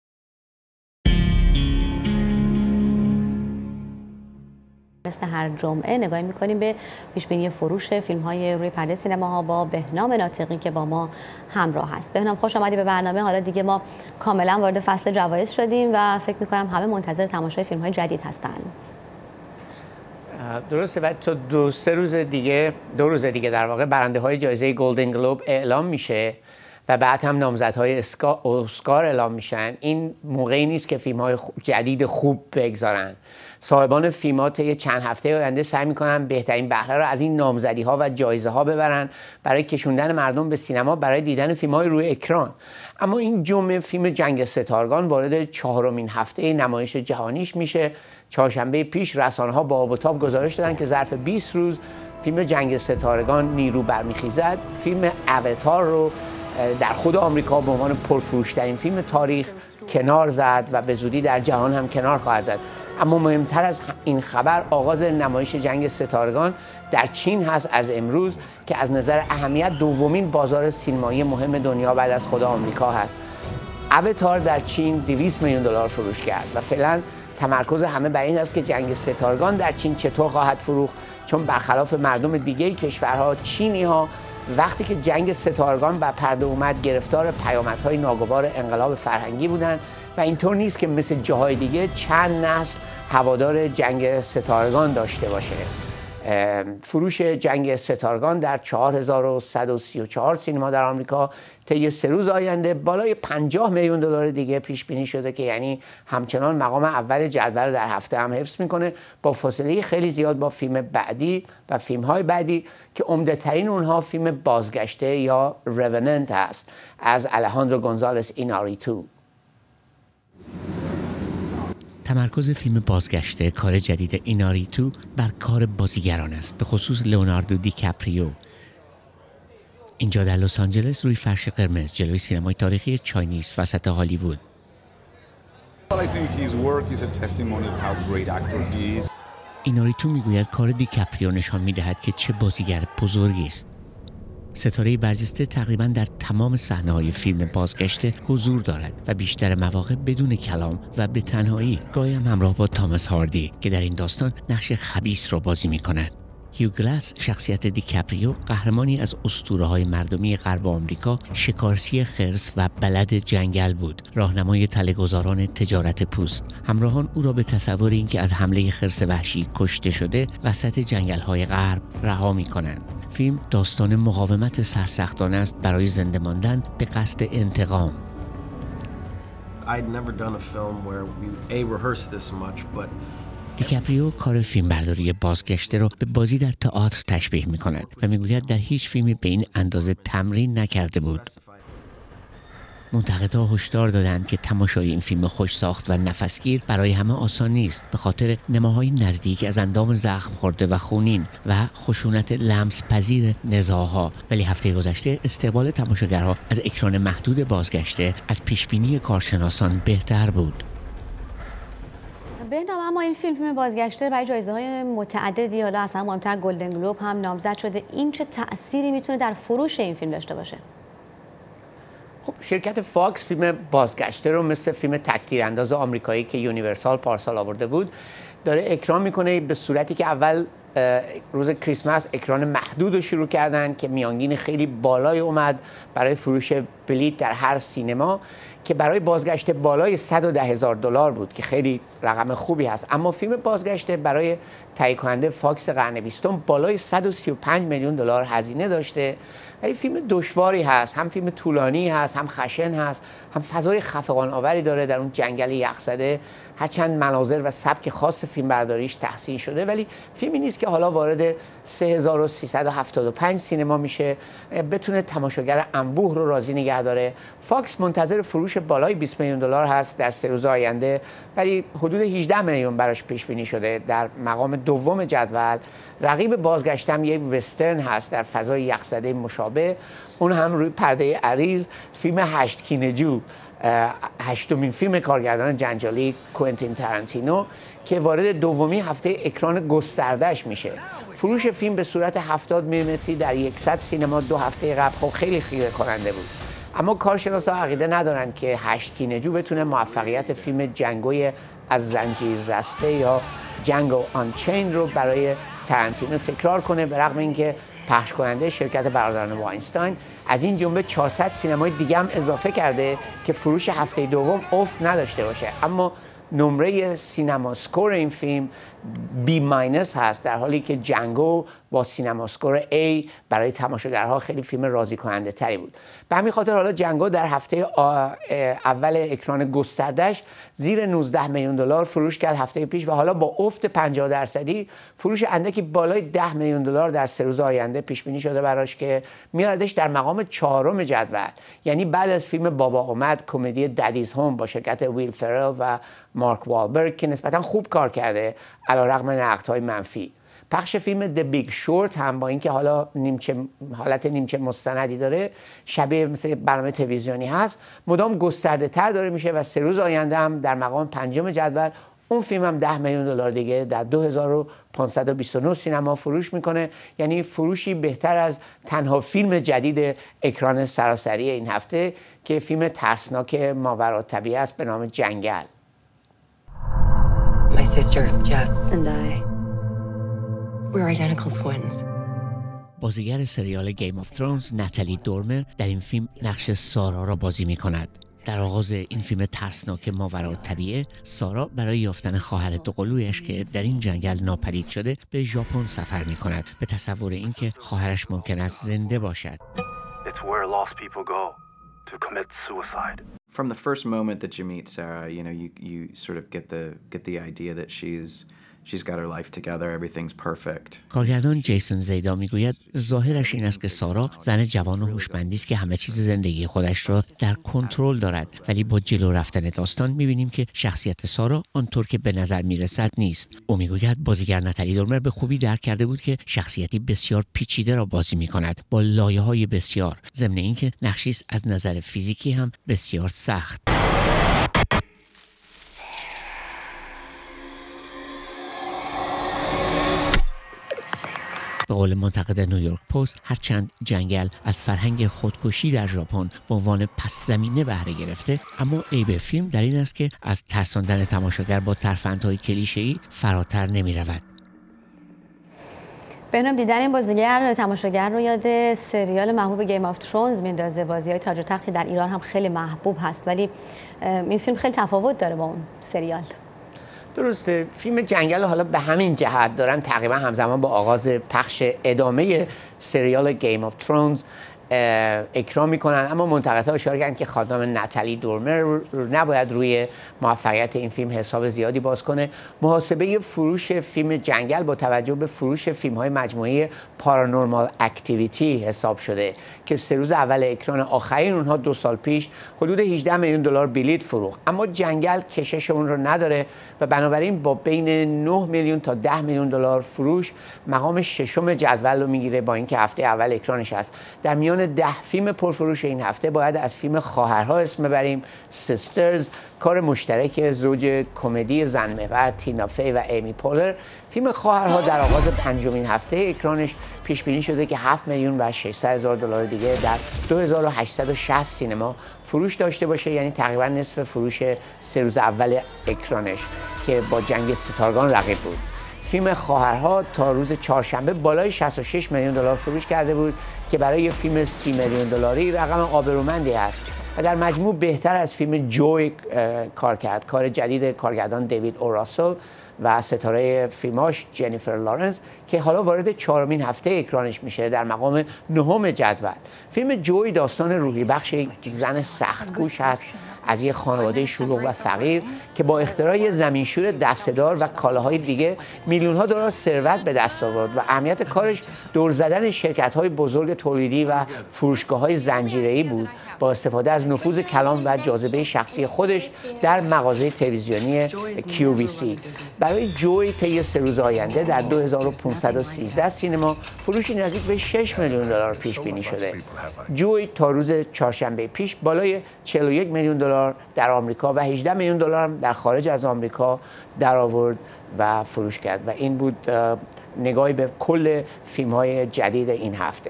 جنگ ستارگان رکورد آواتار را شکست
"نیرو بر می خیزد" آخرین فیلم از مجموعه جنگ ستارگان این هفته با ۷۶۴ میلیون دلار فروش، مقام پرفروش ترین فیلم تاریخ در بازار آمریکا را از "آواتار" ربود. طی سه روز آینده برای آن۵۰ میلیون دلار دیگر فروش در آمریکا پیش بینی شده است.گزارش بهنام ناطقی